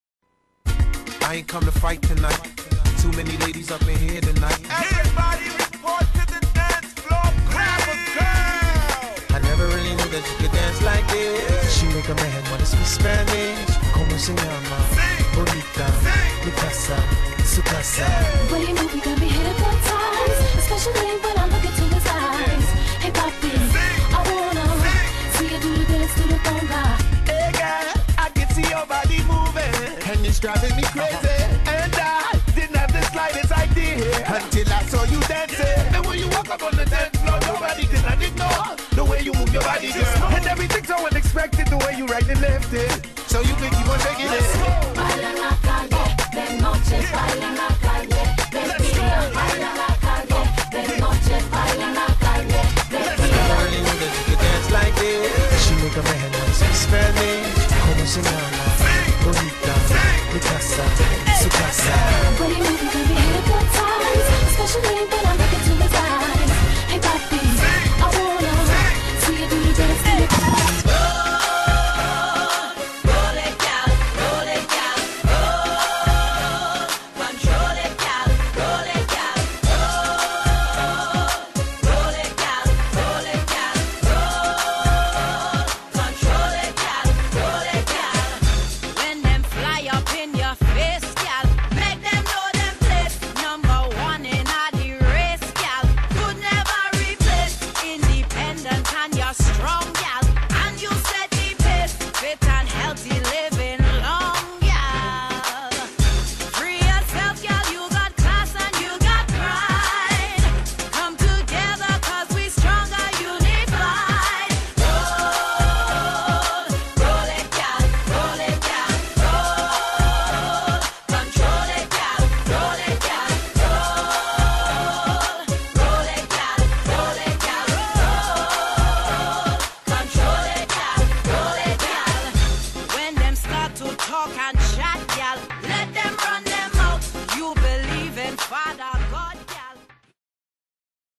DJ Rye LIVE @ Dantes Dance Bar - Windsor, Ontario
Video clip of DJ Rye in action @ Dantes Dance Bar in Windsor Ontario Canada.

You can check him spinning the tunes at Peppers Bar & Grill as well as Dantes Dance Bar every weekend and some weekdays.